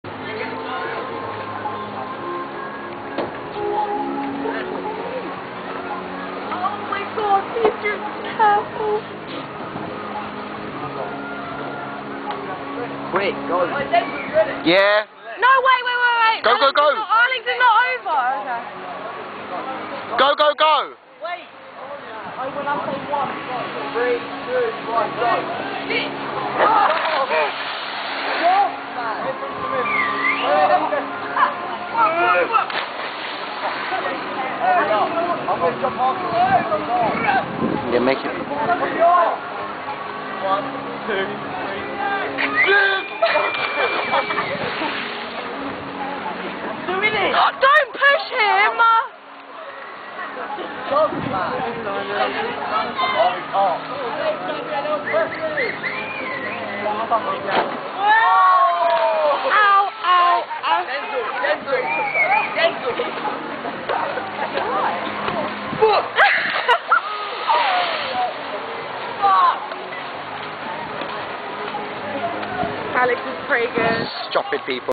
Jumping Of Brighton Pier
Me and some mates jumping of brighton pier!!

pretty lol

Connor and chris jump first
Robbie jumps Third
Luke jumps fourth
Neelen, the fatty jumps 5th and breaks the side.
And im the last guy to fly off one handed